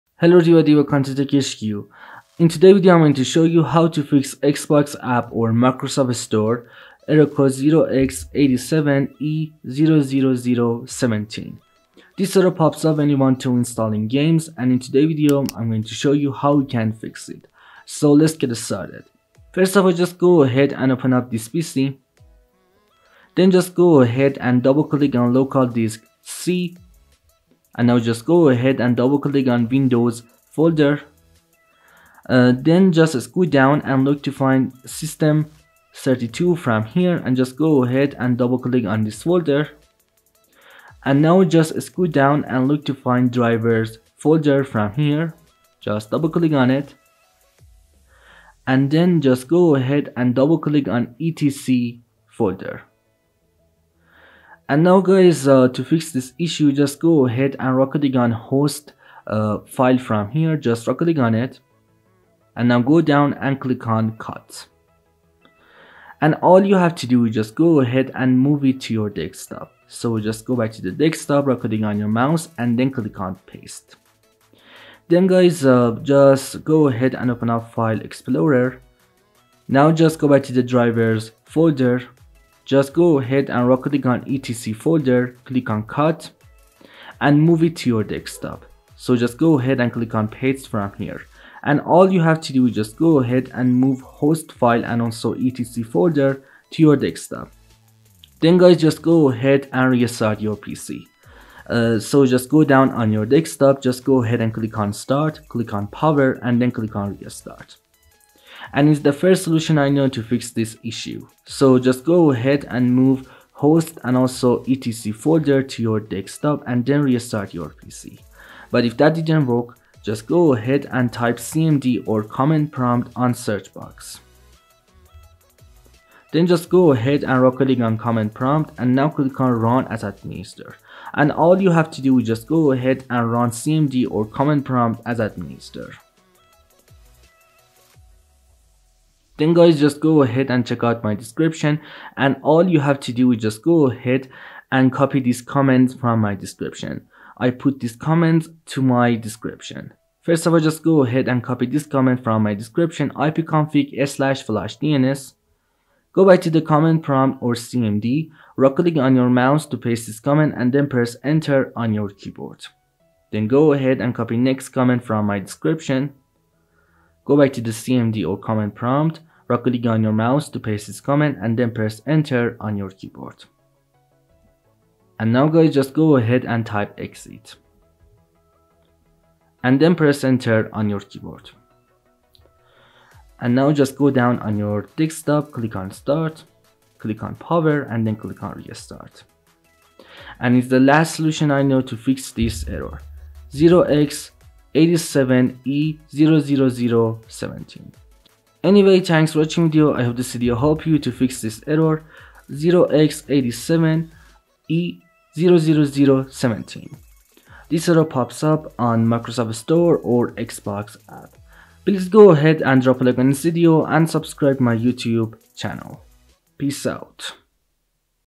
How To Fix Xbox App / Microsoft Store Error Code 0x87e00017 in 2023 | Xbox Gamepass Error 0x87e00017
Hey everybody in this video I'm showing you How To Fix Xbox App / Microsoft Store Error Code 0x87e00017 in 2023 | Xbox Gamepass Error 0x87e00017. so if you want to know how to fix xbox game pass error code 0x87e00017 while installing games just watch this video.

Commands:
1 :ipconfig /flushdns
2 :netsh Winsock reset

0:00 Intro
0:19 Solution 1
2:00 Solution 2
3:18 Outro